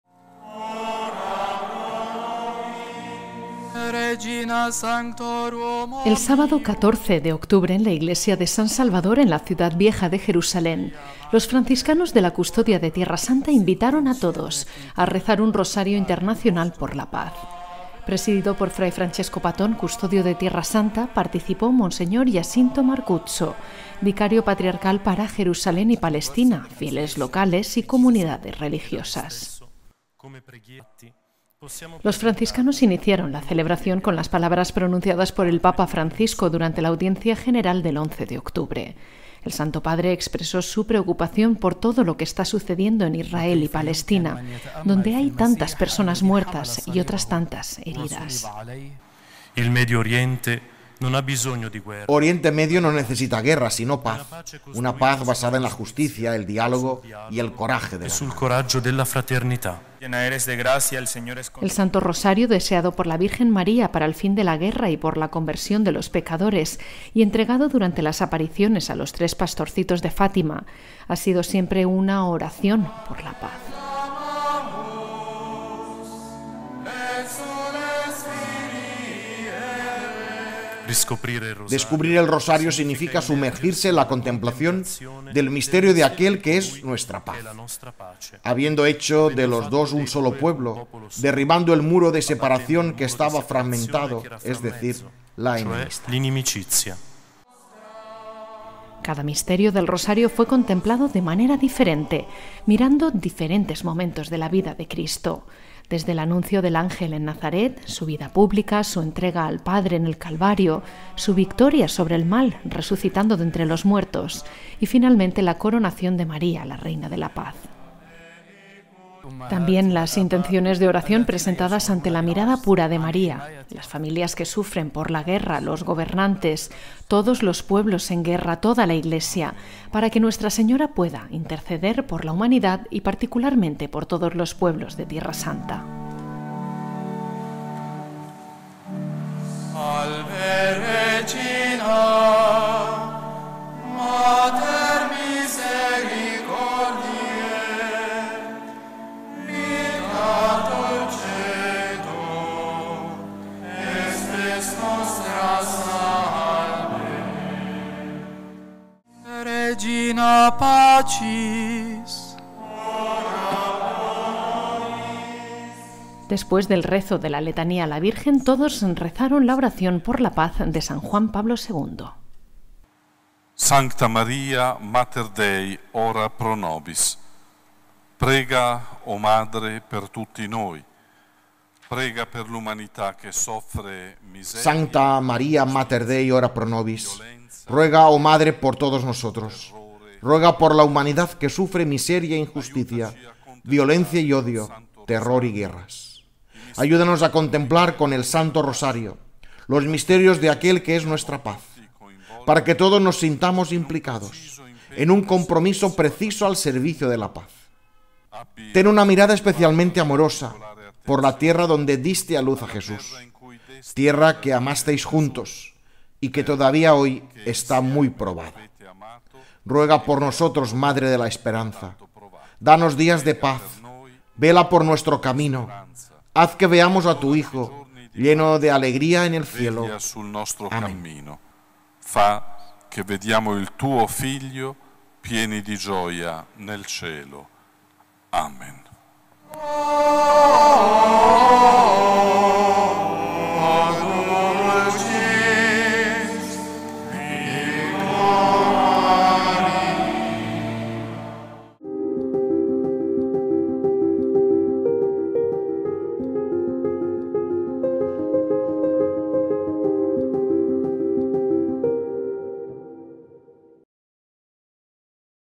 La Custodia de Tierra Santa reza por la paz
Los franciscanos de la Custodia, fieles locales y comunidades religiosas presentes en Tierra Santa rezaron juntos el Santo Rosario por la paz, el sábado 14, en la Iglesia de San Salvador.